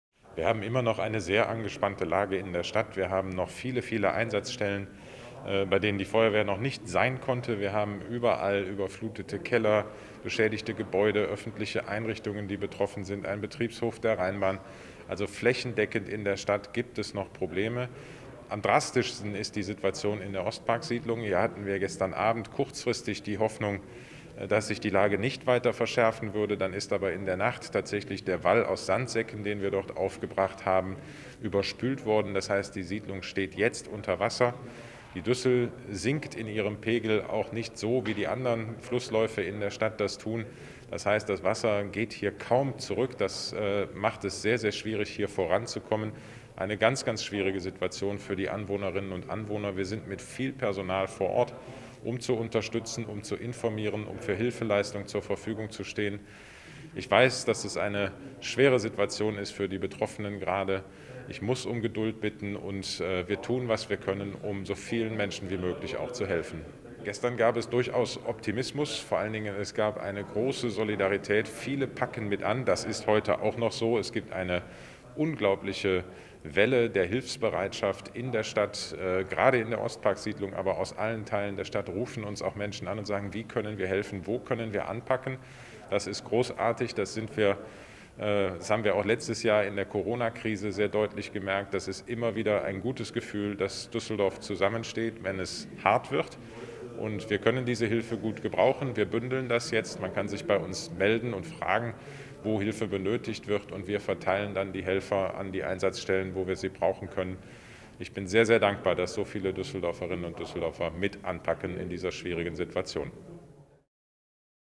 Düssel-Hochwasser: Hilfsprogramm der Stadt läuft auf Hochtouren
Das Düssel-Hochwasser hat in der Nacht zum Donnerstag die Ostpark-Siedlung in Gerresheim geflutet. Wenn Sie Hilfe brauchen oder helfen möchten: Die Versorgungs-Hotline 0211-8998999 des Amtes für Soziales unterstützt Bürgerinnen und Bürger in der Hochwasser-Situation. Das Versorgungstelefon ist montags bis freitags von 9 bis 16 Uhr besetzt und koordiniert Hilfsangebote und -gesuche. Hilfen können die direkte Versorgung betreffen, aber auch Schlafgelegenheiten und Unterkünfte sein, etwa für Düsseldorferinnen und Düsseldorfer, deren Häuser evakuiert werden mussten. Oberbürgermeister Dr. Stephan Keller hat sich für  die große Solidarität der Düsseldorferinnen und Düsseldorfer bedankt.